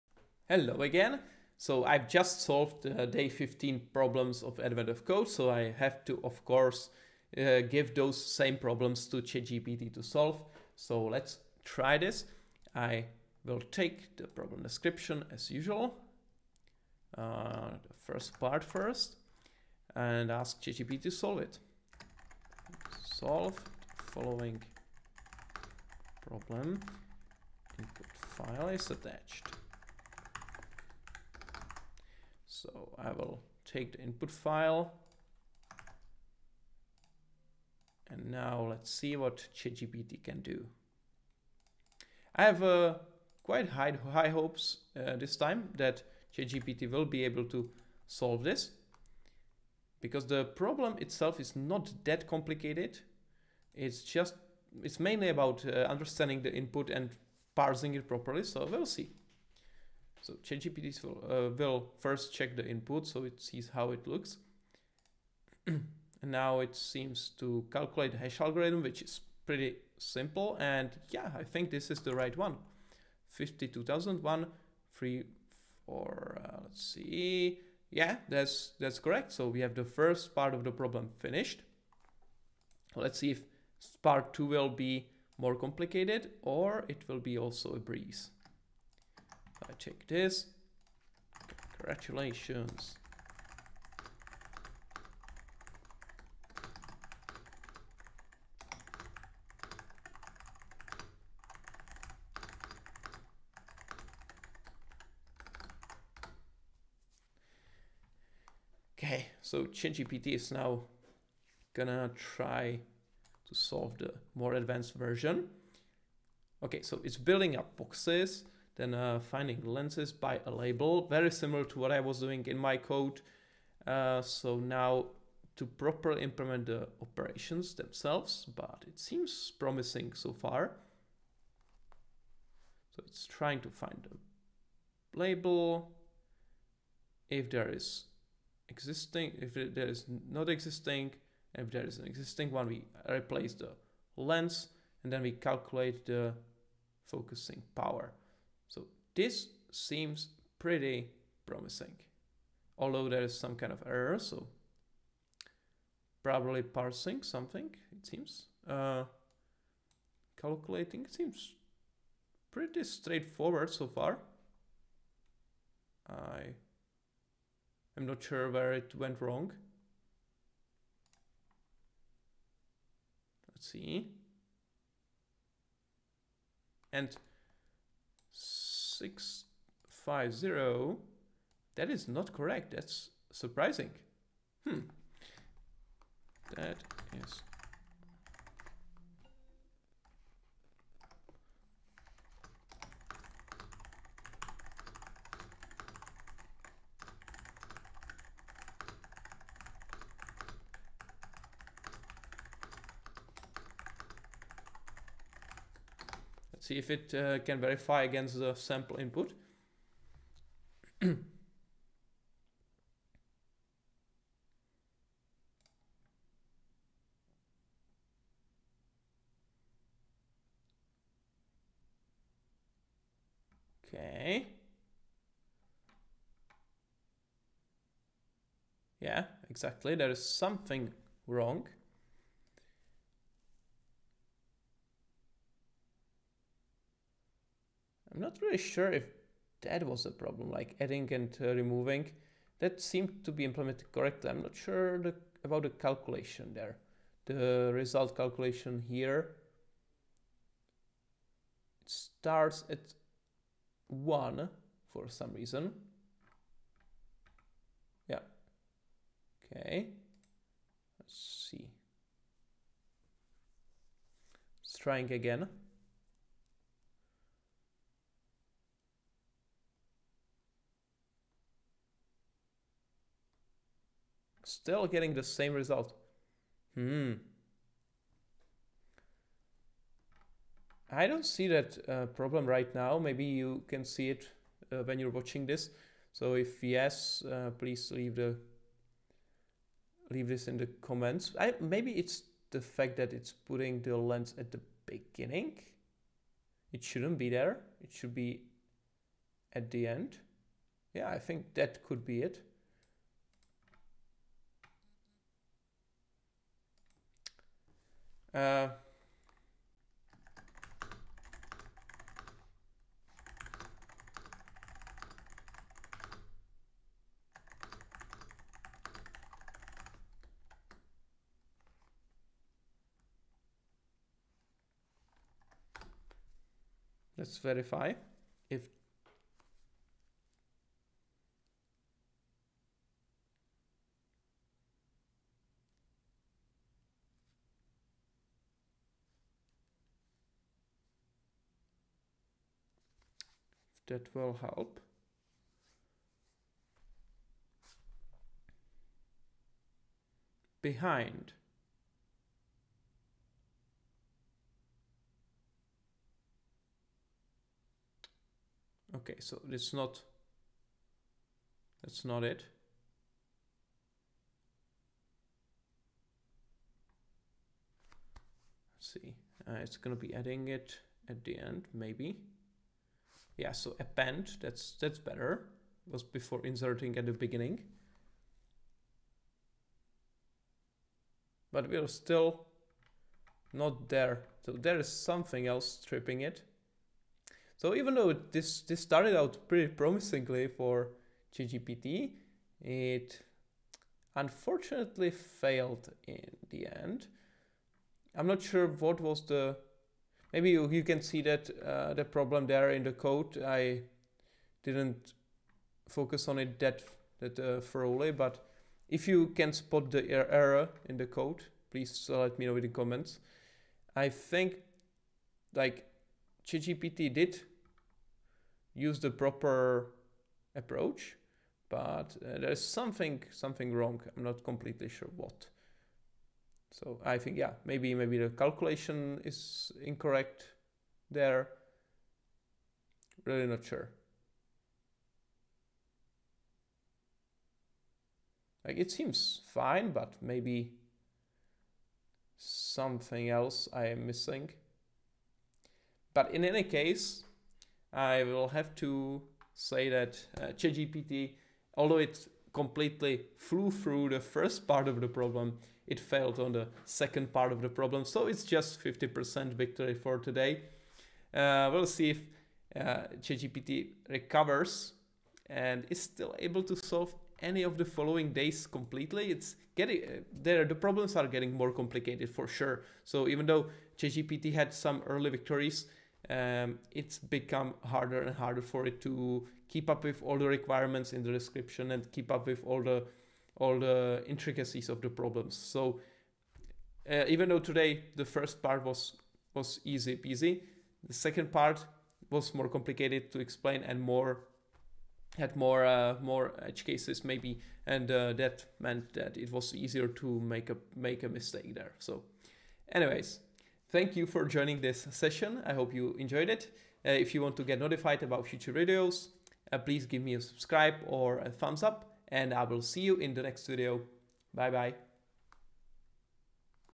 ChatGPT vs. Advent of Code 2023 Day 15: Lens Library
Will ChatGPT rise from the ashes and solve day 15? Let's find out!

Contents:
0:00 - Intro
0:13 - Part 1
1:35 - Part 2
7:30 - Summary